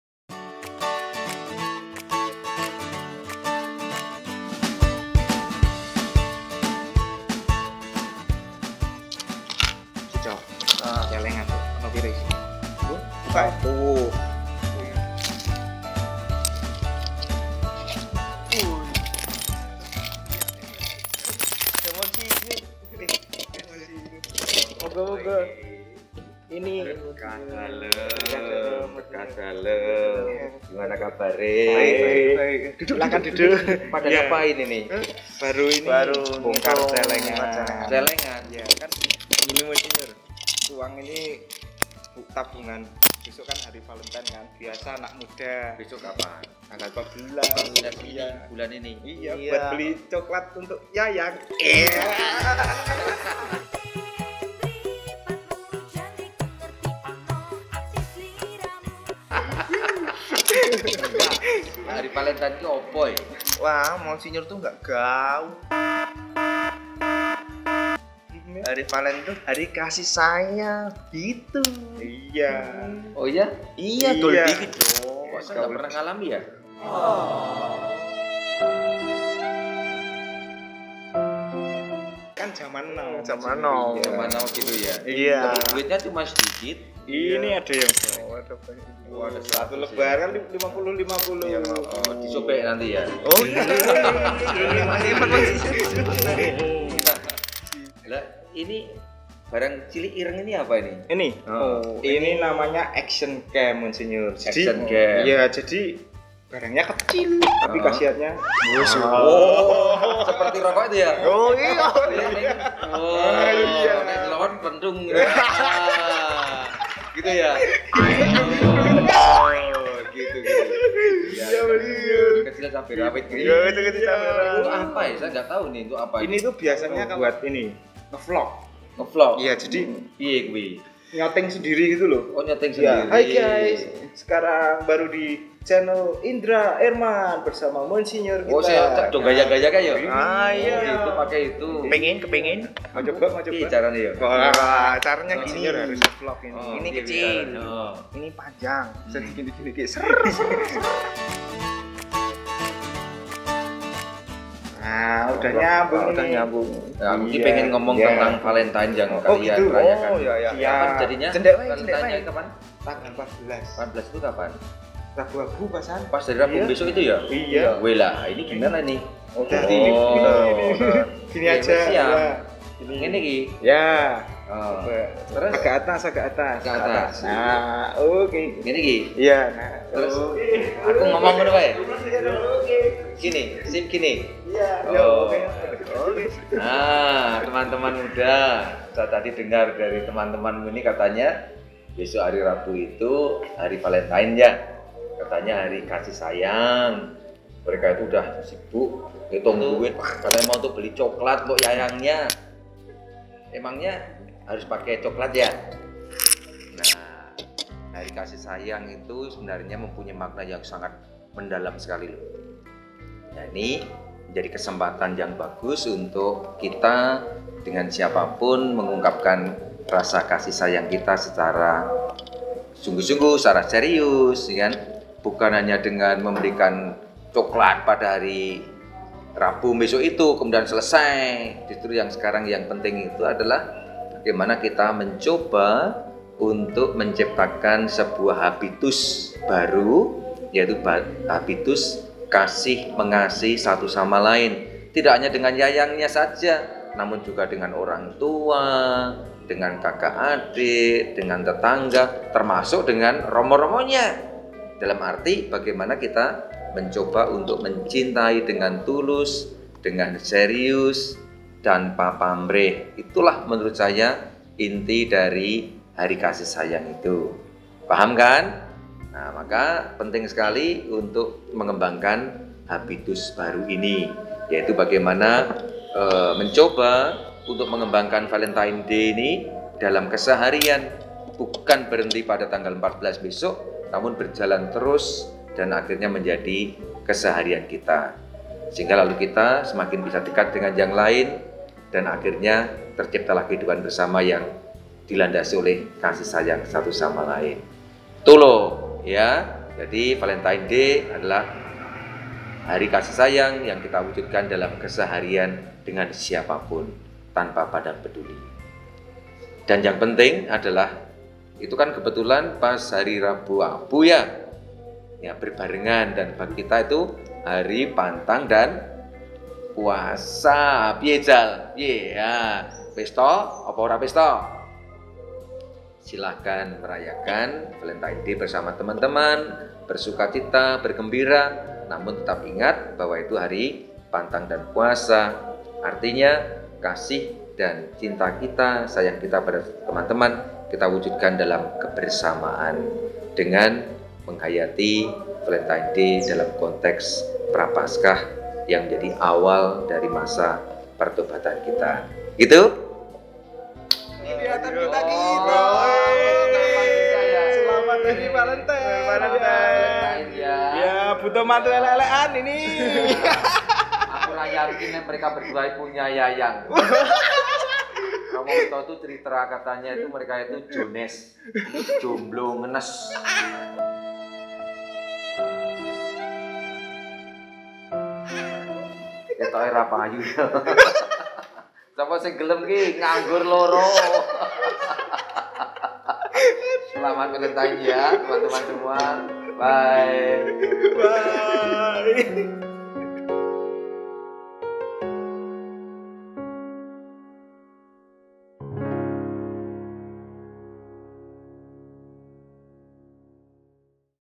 ErmanxIndro the series episode: Mgr. Rubi: Selamat merayakan hari kasih sayang,.
Hidupilah hari kasih sayang setiap hari agar tercipta kehidupan bersama yang dilandasi oleh kasih-sayang satu sama lain. Mari saling menyayangi, karena jika kasih sayang memenuhi diri tidak akan ada tempat untuk rasa benci.